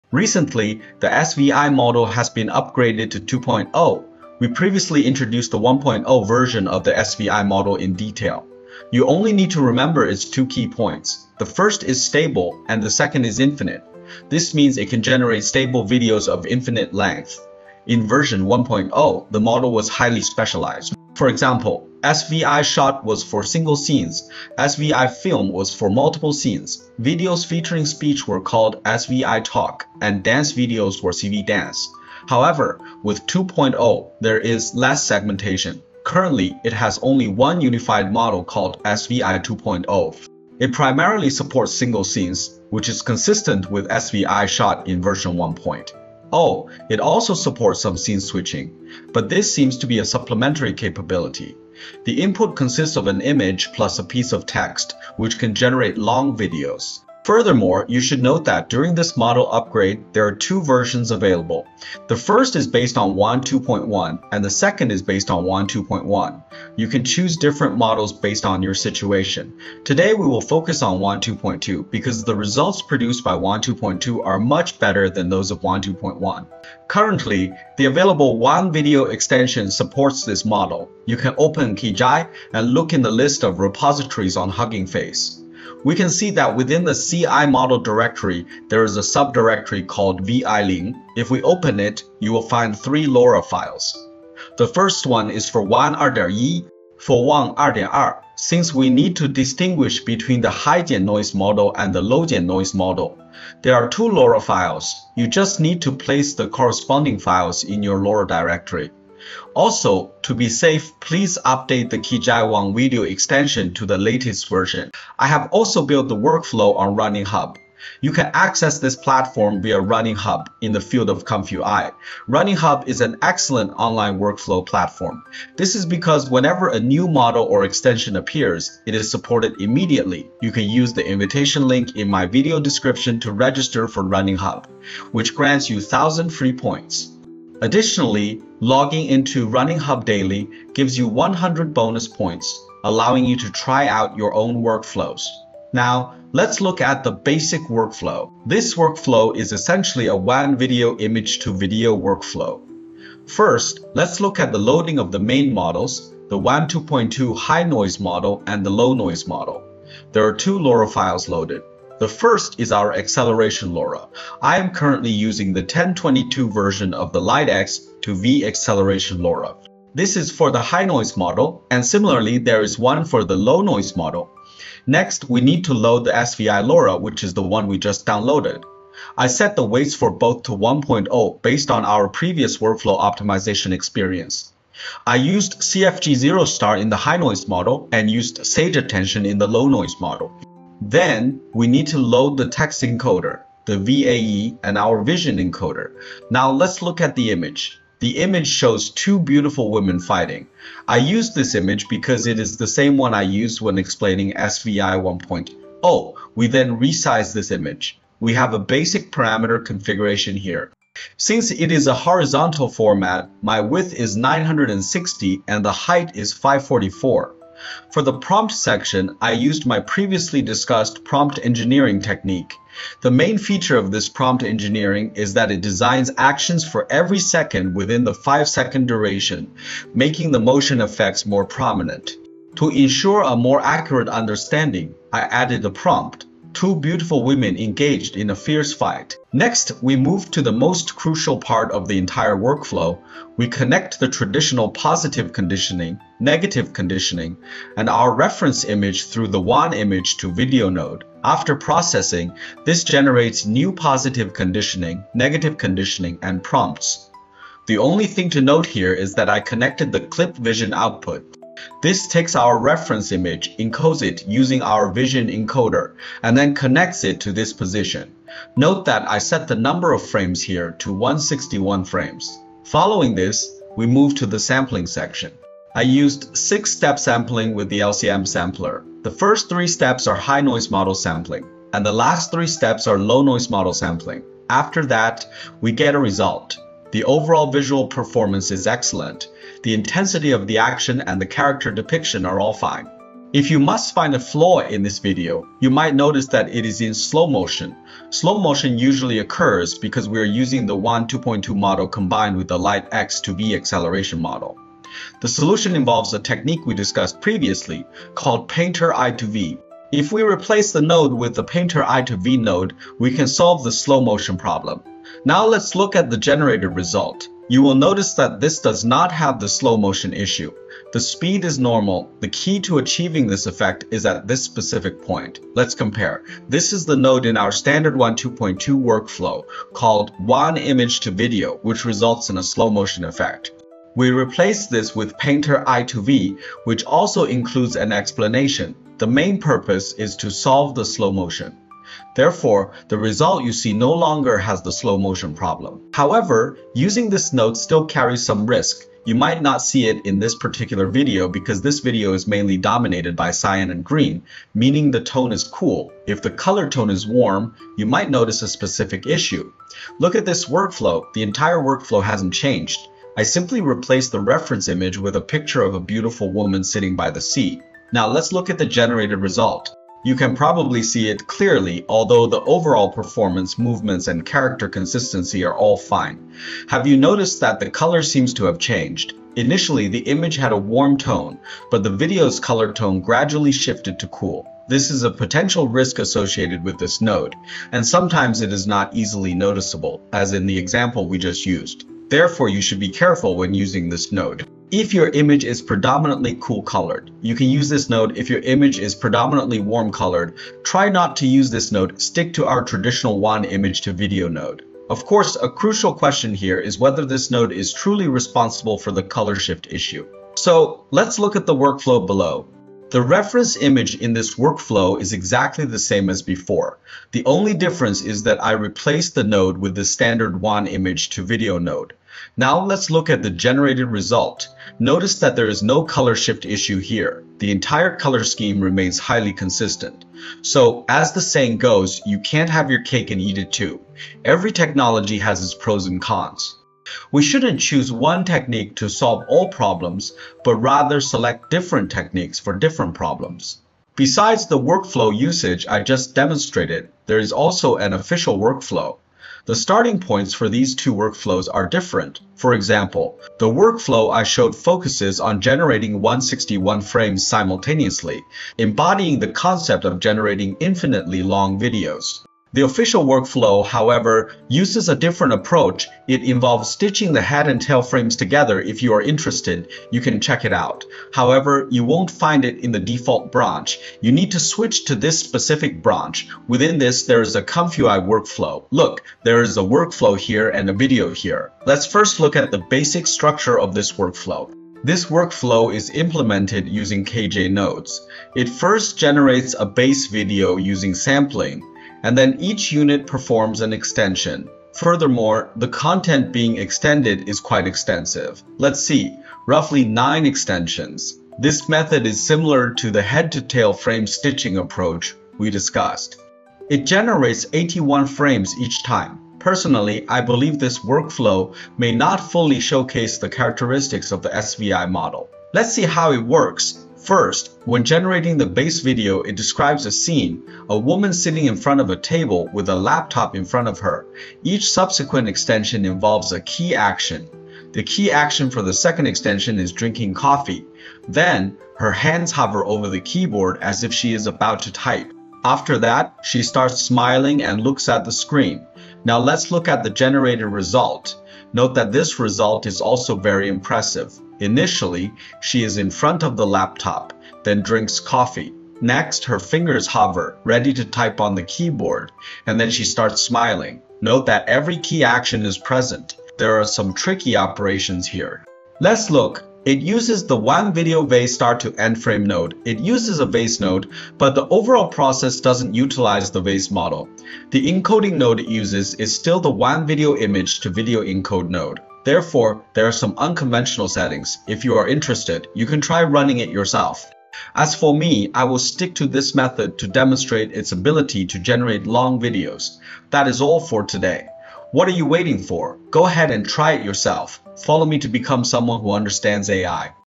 Generate Infinite AI Video! SVI 2.0 Workflow Guide|Fix Slow Motion & Color Shift: SVI 2.0
SVI (Stable Video Infinity) has upgraded to version 2.0! In this video, we explore how SVI 2.0 combined with Wan 2.2 allows for the generation of stable, infinite-length videos. Unlike version 1.0, the new 2.0 model is unified and much easier to use.
I will walk you through the complete ComfyUI workflow, showing you how to set up the High/Low Noise models, apply the Light X2V acceleration LoRA, and generate complex action scenes. We also dive into a crucial fix for "slow-motion" issues using the Painter I2V node—and why you need to be careful with it (watch out for the color shift bug!). Finally, we look at the official Kijai workflow for creating seamless long-video extensions.
📌 Key Topics Covered:
SVI 2.0 (Stable Video Infinity): The new unified model architecture.
Setup: Wan 2.2 High/Low Noise models + SVI 2.0 LoRA weights.
Speed Up: Using Light X2V acceleration (1022 version).
Troubleshooting: Fixing slow motion with Painter I2V & avoiding color temperature shifts.

try the workflow online
1: video_wan2_2_14B_i2v_svi2